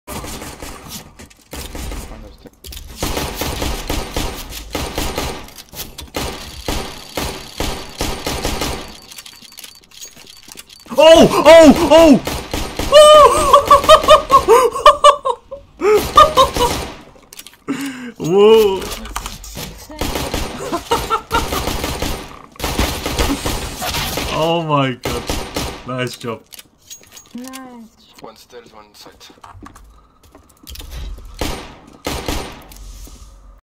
EZ Knife
I play with random russians and I get a knife. Yes I am a man child that is suprised by pixels.